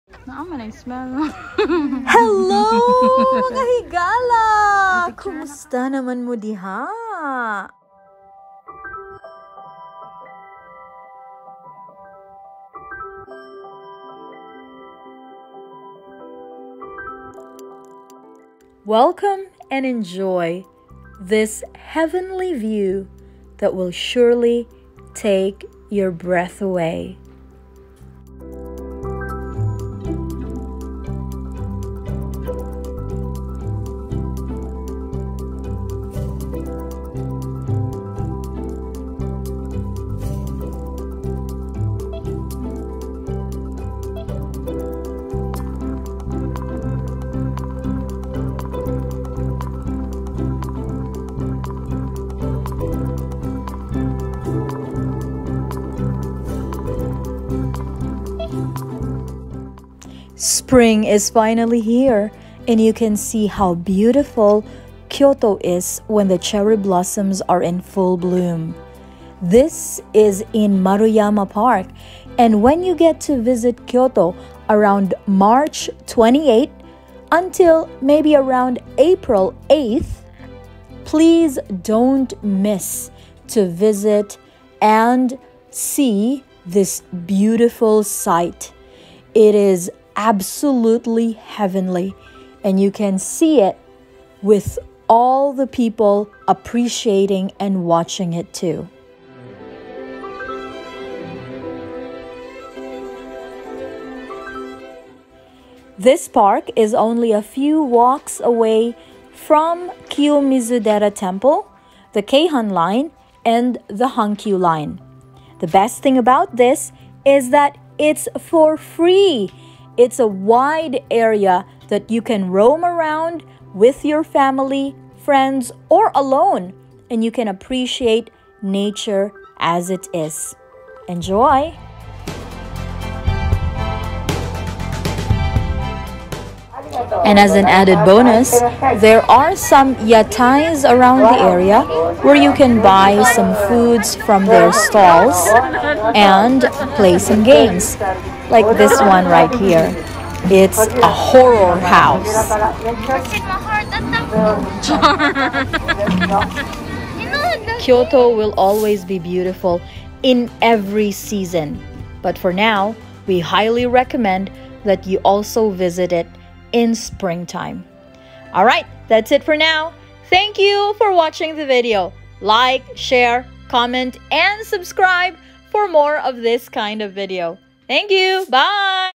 KYOTO CHERRY BLOSSOMS in FULL BLOOM for 2022 in Maruyama Park
Japan Life for a Filipino Family... another magical experience as we enjoy and appreciate the beauty of Kyoto specifically in Maruyama Park!